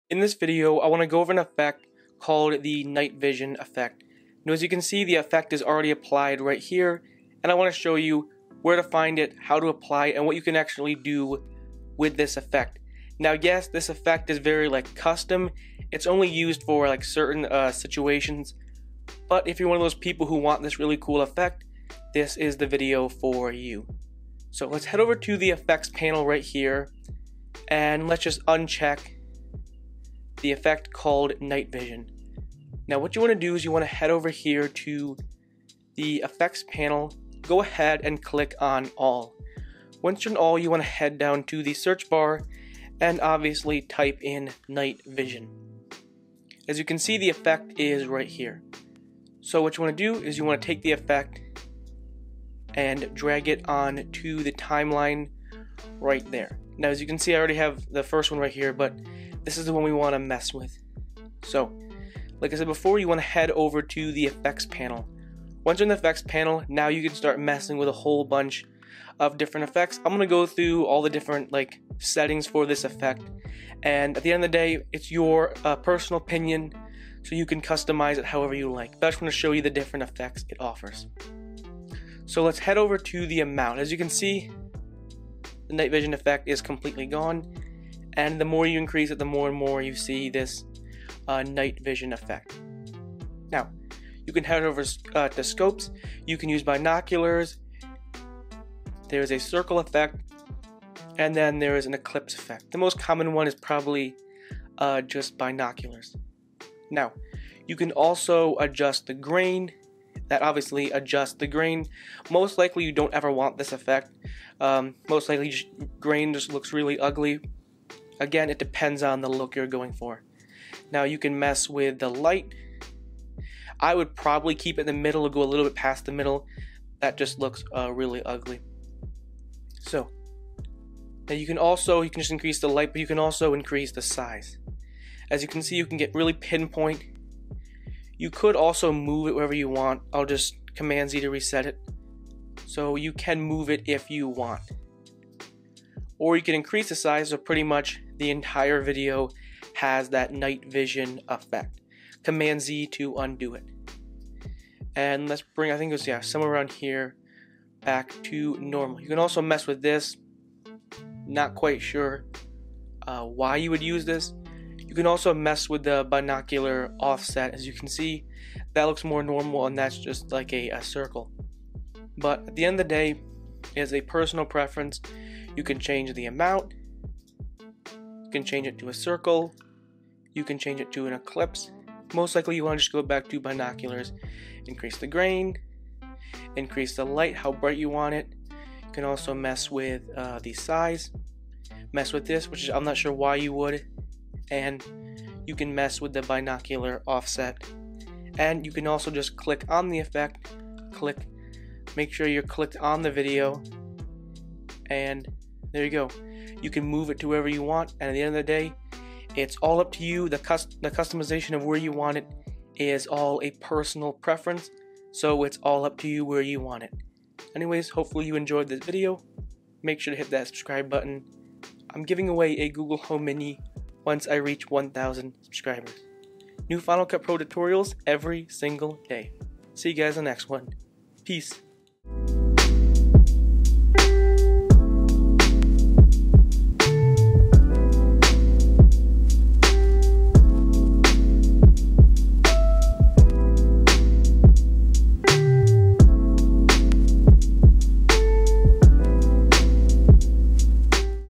Final Cut Pro X Night Vision Effect Tutorial
In this tutorial, I will break down how to add a night vision effect to your videos using Final Cut Pro X.

Desktop - iMac (Retina 4K, 21.5-inch, 2017)